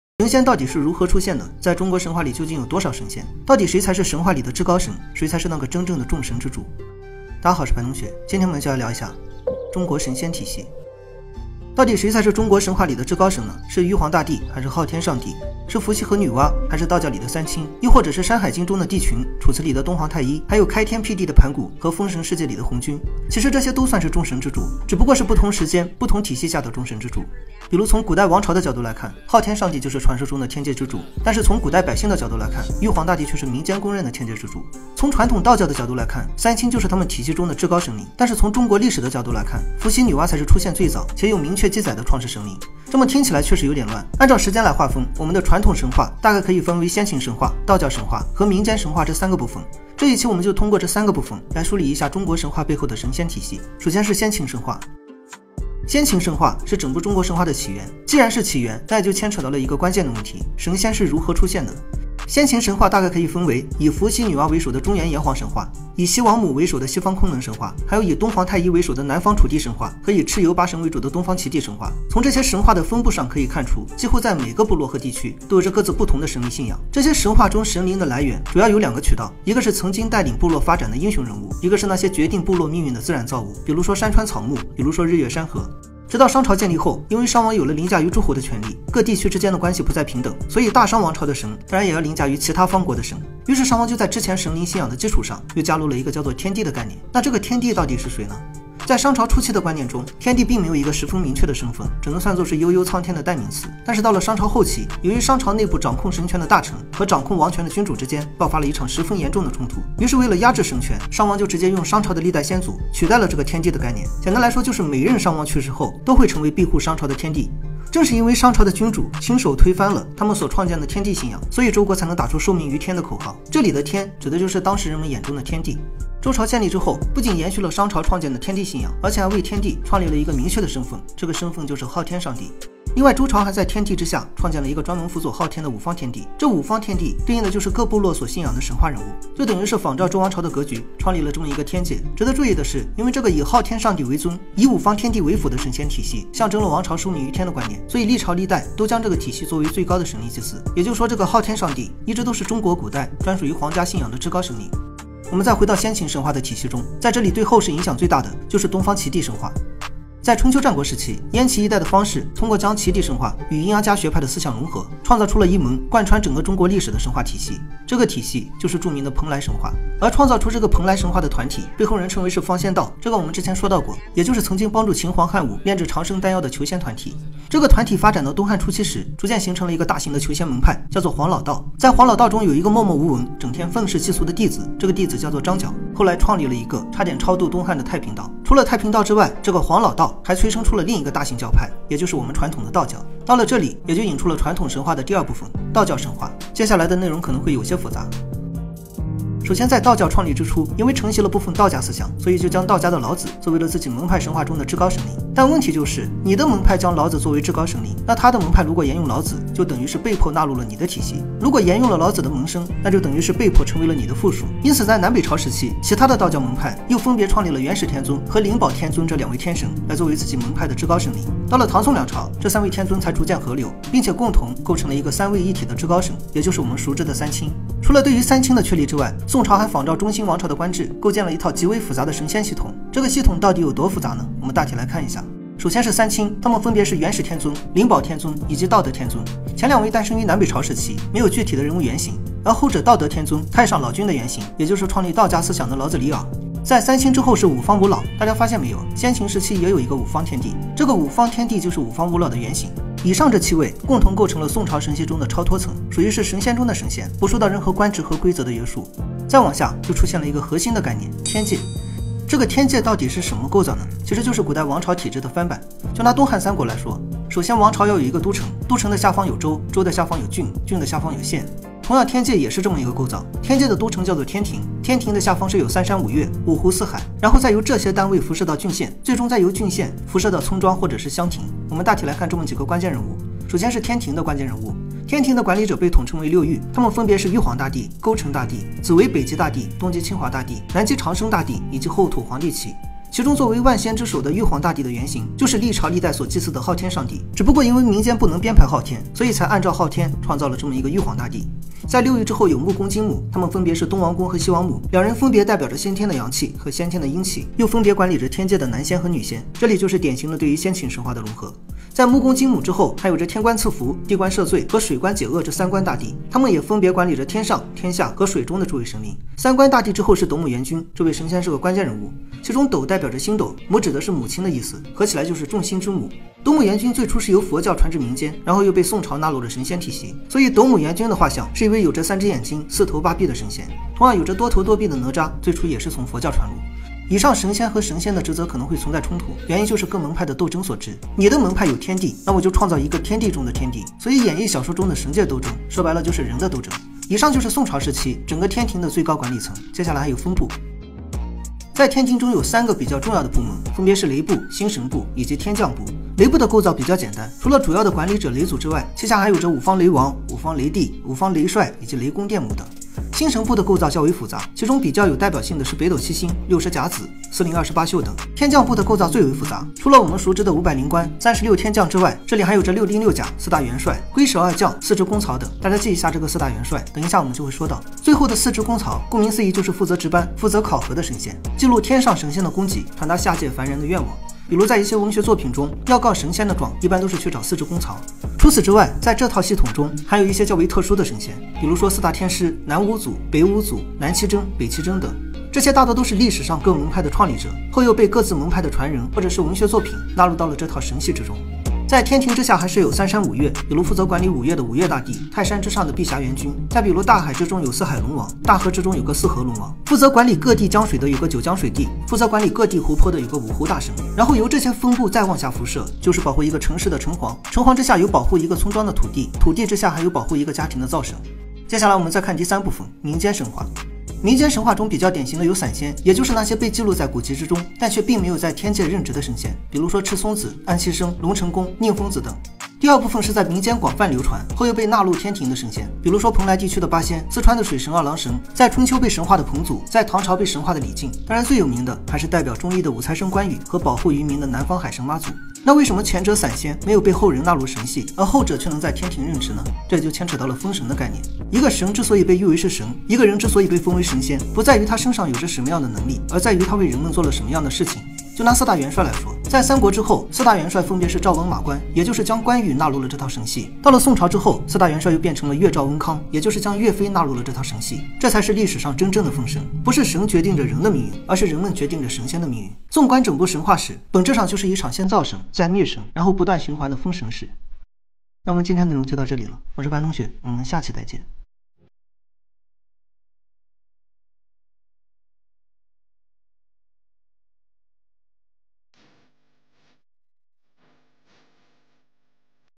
神仙是如何出现的？中国神话里有多少神仙？人到底是如何封神的
你的白同学上线！
你想了解奇闻异事吗？想知道事情的真相吗？
硬核传奇和硬核案件，这里，全部都是奇闻……
令你脑洞大开